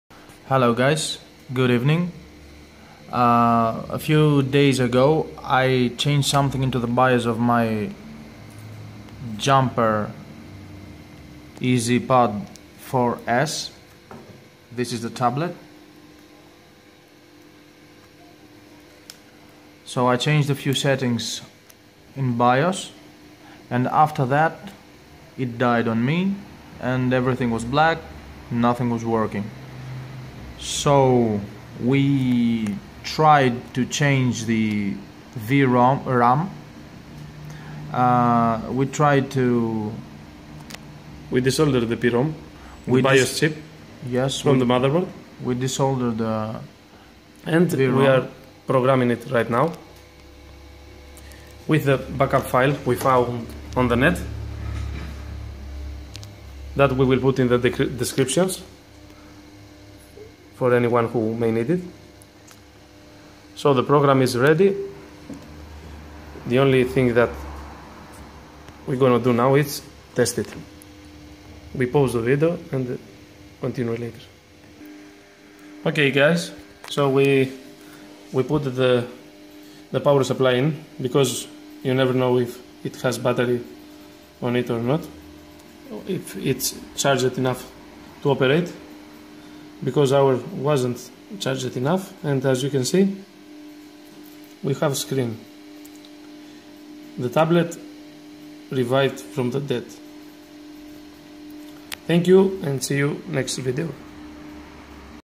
Jumper EzPad 4s Tablet Dead Fix
This is how you can fix jumper ezpad 4s tablet died after changing bios settings.